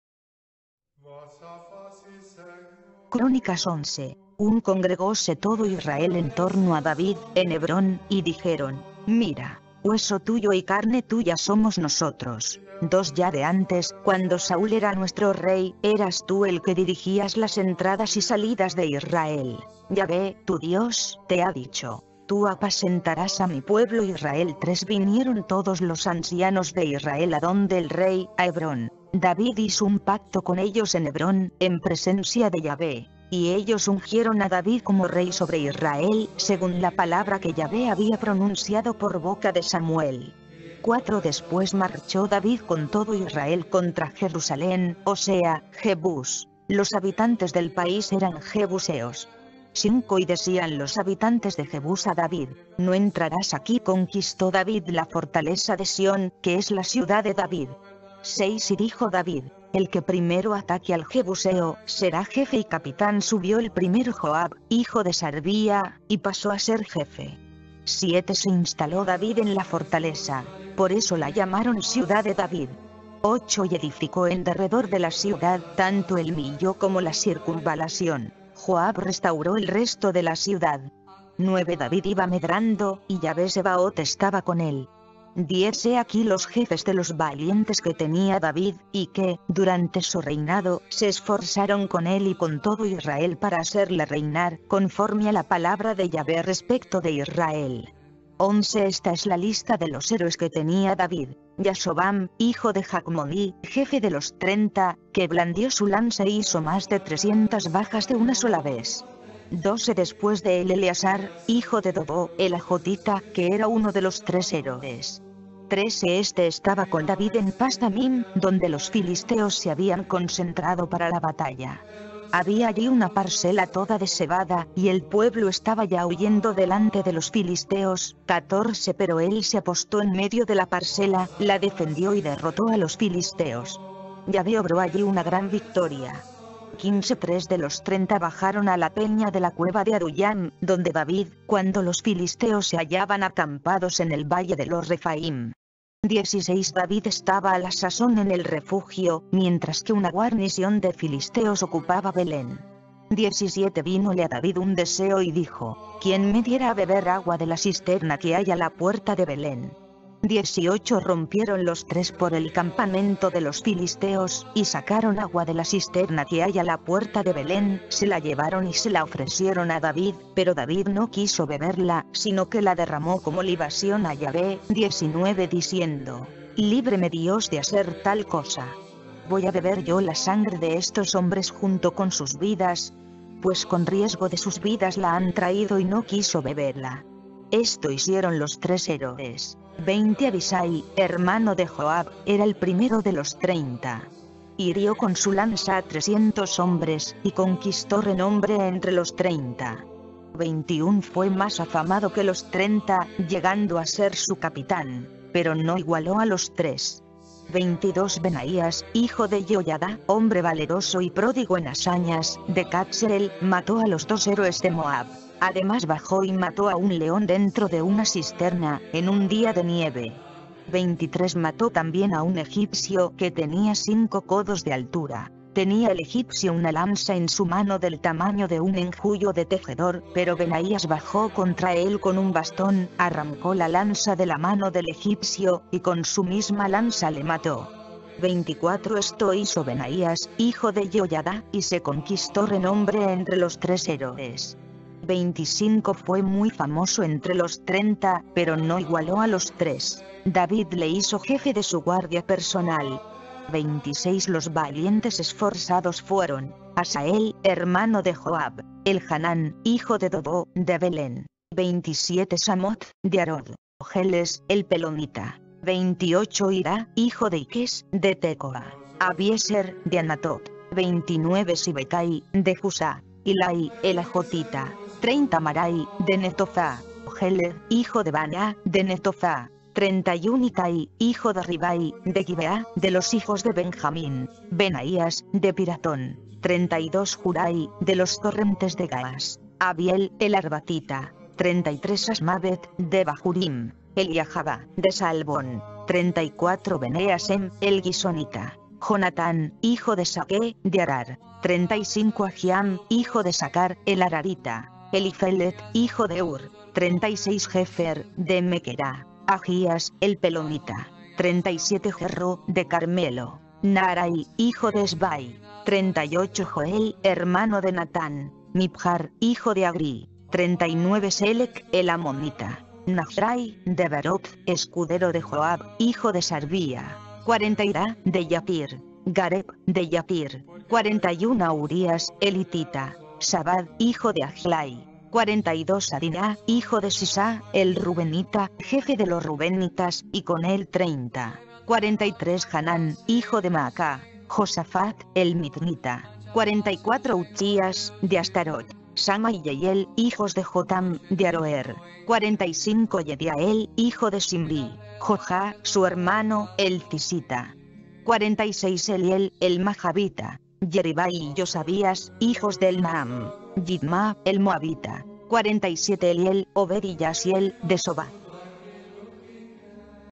LA SANTA BIBLIA,VERSIÓN BIBLIA DE JERUSALÉN 1976, 1 Crónicas 11
Crónicas 11

1 Congregóse todo Israel en torno a David, en Hebrón, y dijeron: «Mira: hueso tuyo y carne tuya somos nosotros.
2 Ya de antes, cuando Saúl era nuestro rey, eras tú el que dirigías las entradas y salidas de Israel; Yahveh, tu Dios, te ha dicho: “Tú apacentarás a mi pueblo Israel.”» 3 Vinieron todos los ancianos de Israel adonde el rey, a Hebrón; David hizo un pacto con ellos en Hebrón, en presencia de Yahveh; y ellos ungieron a David como rey sobre Israel, según la palabra que Yahveh había pronunciado por boca de Samuel. 4 Después marchó David con todo Israel contra Jerusalén, o sea, Jebús; los habitantes del país eran jebuseos. 5 Y decían los habitantes de Jebús a David: «No entrarás aquí.» Conquistó David la fortaleza de Sión, que es la Ciudad de David. 6 Y dijo David: «El que primero ataque al jebuseo, será jefe y capitán.» Subió el primero Joab, hijo de Sarvia, y pasó a ser jefe. 7 Se instaló David en la fortaleza; por eso la llamaron Ciudad de David. 8 Y edificó en derredor de la ciudad, tanto el Milló como la circunvalación; Joab restauró el resto de la ciudad. 9 David iba medrando, y Yahveh Sebaot estaba con él. 10 He aquí los jefes de los valientes que tenía David, y que, durante su reinado, se esforzaron con él y con todo Israel para hacerle reinar, conforme a la palabra de Yahveh respecto de Israel. 11 Esta es la lista de los héroes que tenía David: Yasobam, hijo de Jakmoní, jefe de los Treinta, que blandió su lanza e hizo más de trescientas bajas de una sola vez. 12 Después de él Eleazar, hijo de Dodó, el ajotita, que era uno de los Tres héroes. 13 Este estaba con David en Pas Dammim, donde los filisteos se habían concentrado para la batalla. Había allí una parcela toda de cebada, y el pueblo estaba ya huyendo delante de los filisteos, 14 pero él se apostó en medio de la parcela, la defendió y derrotó a los filisteos. Yahveh obró allí una gran victoria. 15 Tres de los Treinta bajaron a la peña de la cueva de Adullam, donde David, cuando los filisteos se hallaban acampados en el valle de los Refaím. 16 David estaba a la sazón en el refugio, mientras que una guarnición de filisteos ocupaba Belén. 17 Vínole a David un deseo y dijo: «¡Quién me diera a beber agua de la cisterna que hay a la puerta de Belén!» 18 Rompieron los Tres por el campamento de los filisteos, y sacaron agua de la cisterna que hay a la puerta de Belén, se la llevaron y se la ofrecieron a David, pero David no quiso beberla, sino que la derramó como libación a Yahveh,